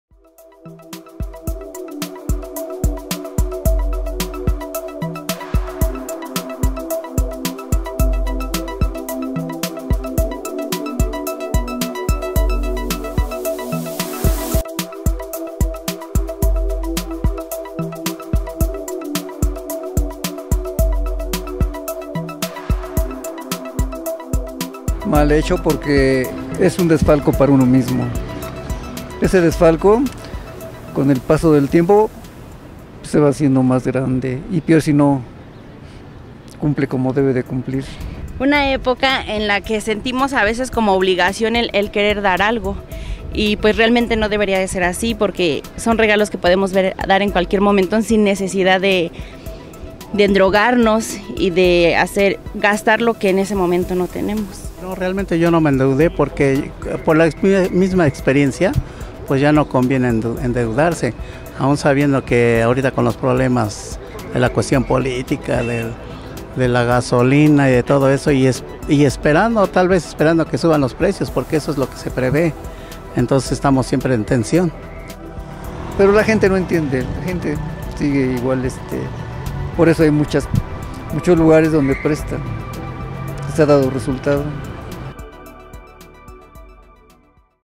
Cuesta de enero 2019
¿Acaso los capitalinos son víctimas de la cuesta de enero?